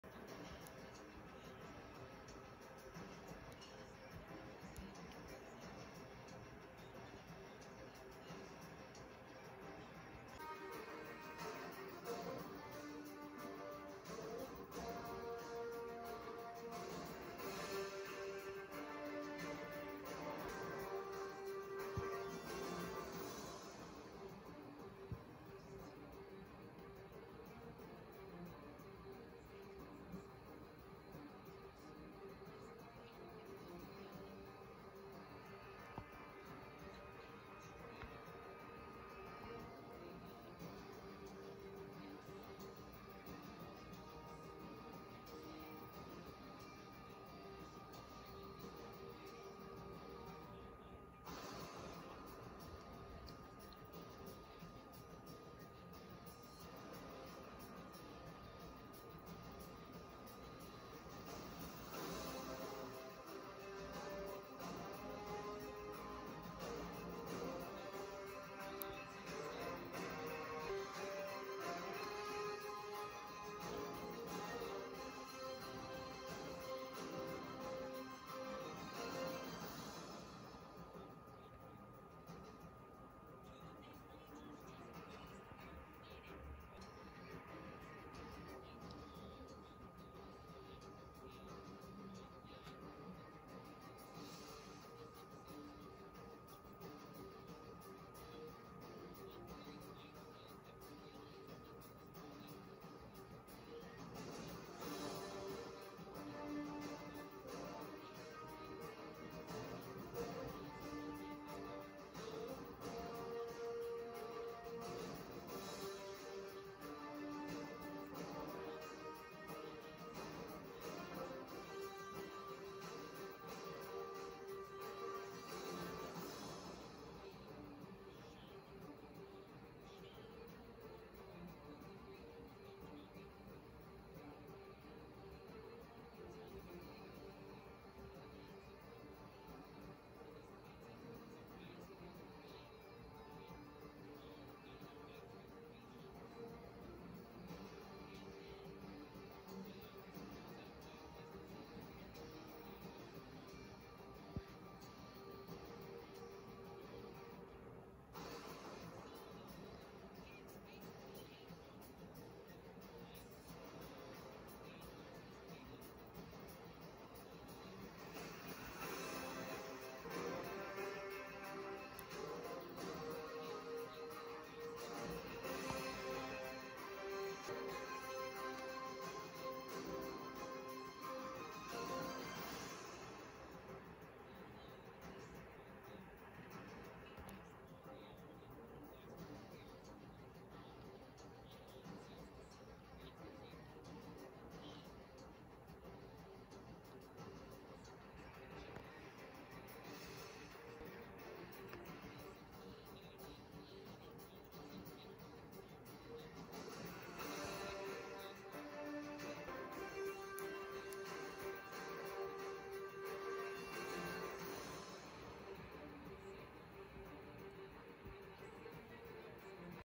NFL Training Games 2023: Arizona Cardinals Stats
These are the stats from the Arizona Cardinals during their 2023 training game.